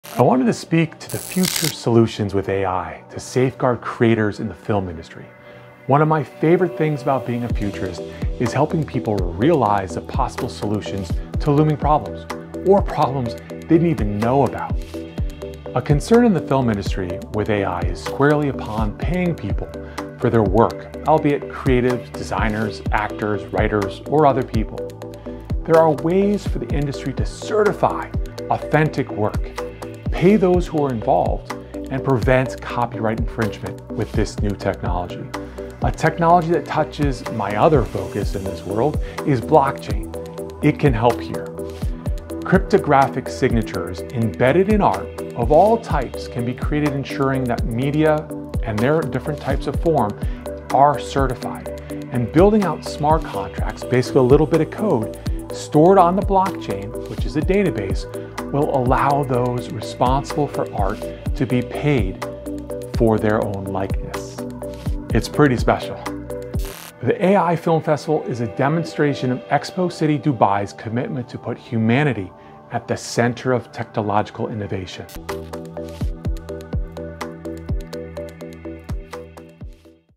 AI in Film and Video - How to Combat Copyright and Ownership
How can you make sure companies and people are paid for their work in film?  I outline a possible solution.